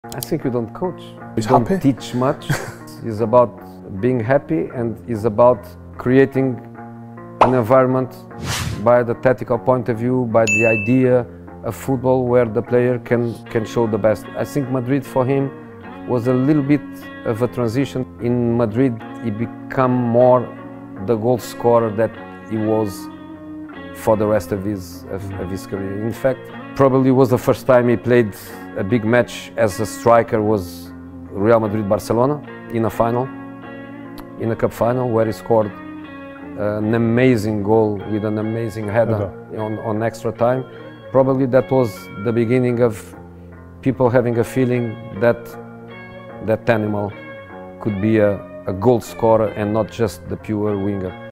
Jose Mourinho On Ronaldo’s Position Change
Jose Mourinho who spent 3 years from 2010 to 2013 with Cristiano Ronaldo at Real Madrid talks about how he did not have to coach Ronaldo but just help set up the team correctly. Mourinho also mentions how he transitioned from winger to striker gradually at Real Madrid.

Credit to @rioferdinandpresents for the interview.